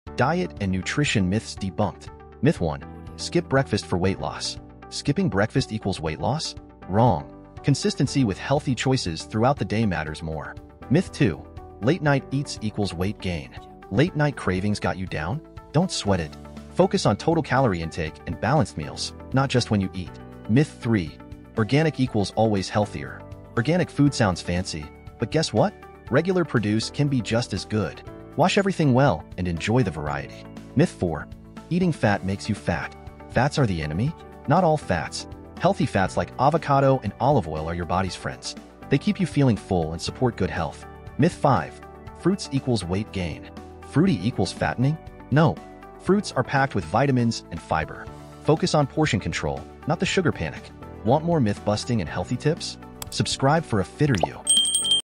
5 Diet Myths You Need to Know.
Dive into the truth behind common diet misconceptions! In this short video, we unravel 5 myths about dieting that might be holding you back from your health goals. From debunking fads to shedding light on misconceptions, get ready to rethink your approach to nutrition!